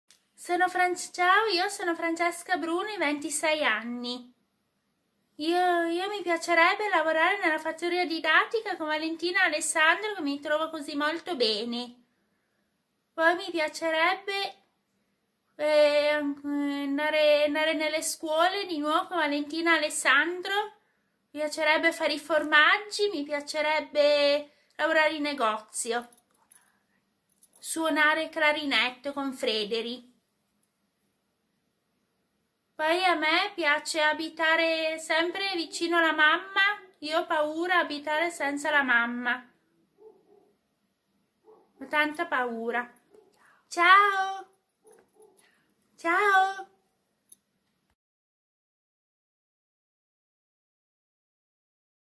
Giornata internazionale delle persone con disabilità | Sogni e desideri - Francesca
In occasione della Giornata internazionale delle persone con disabilità, Francesca racconta i suoi sogni e i suoi desideri.